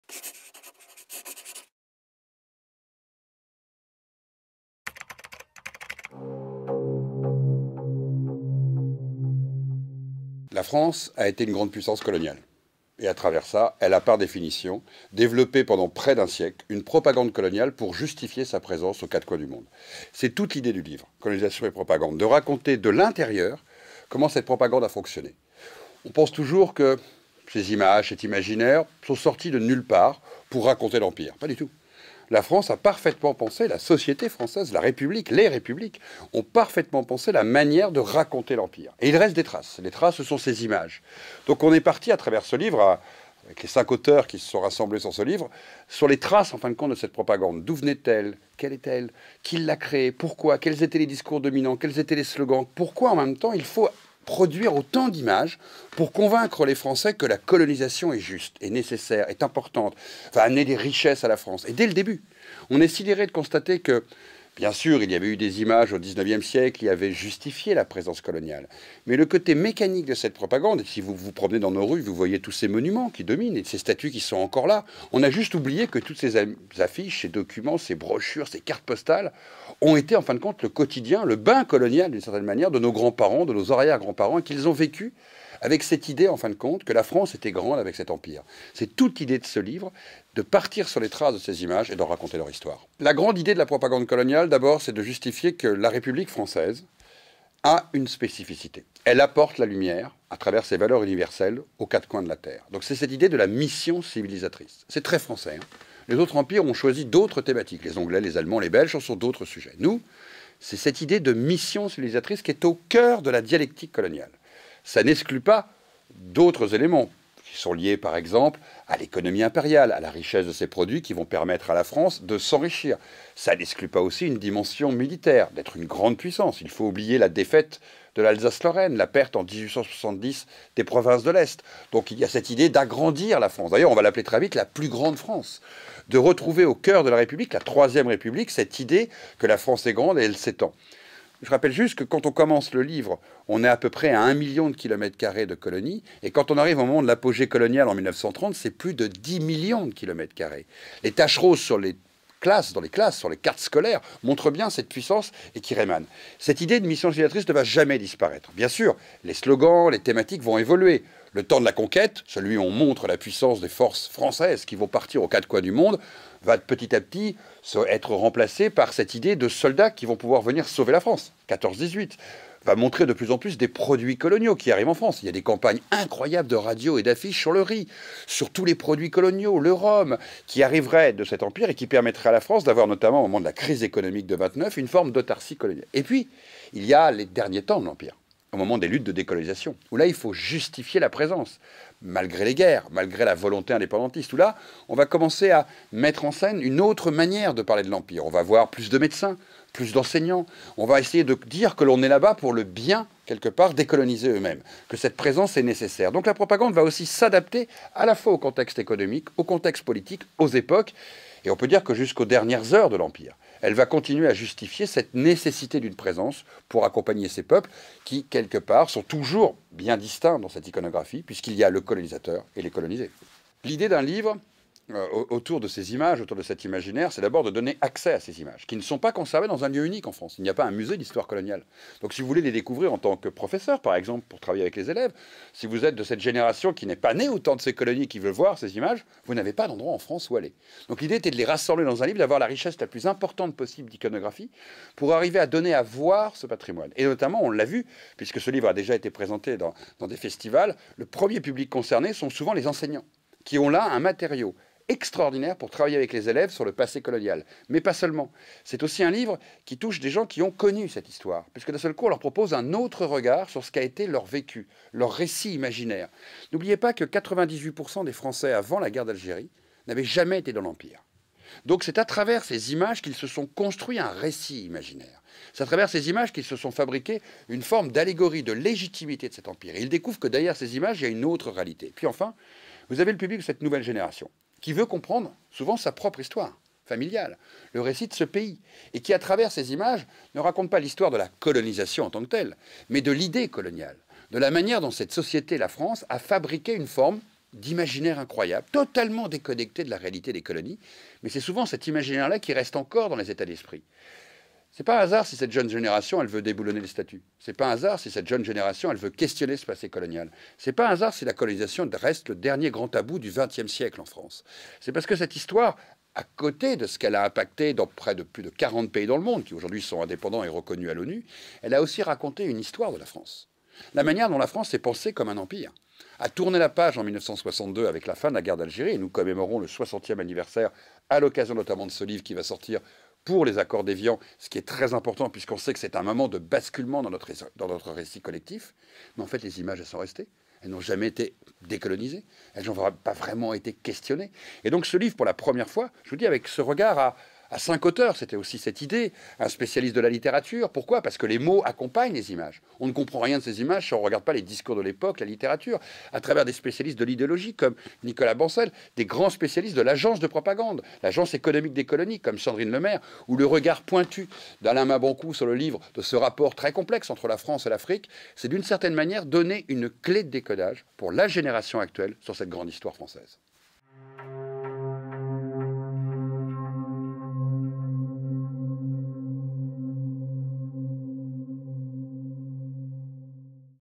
Pascal Blanchard - Colonisation & propagande : le pouvoir de l'image
Pascal Blanchard vous présente l'ouvrage "Colonisation & propagande : le pouvoir de l'image" aux éditions Cherche Midi.